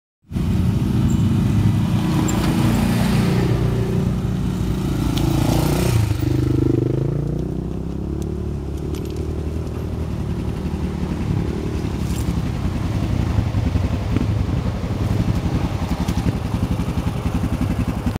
3 °raduno quad Siracusa aspettando i compagni
Renato  giuseppe ke aspettano gli altri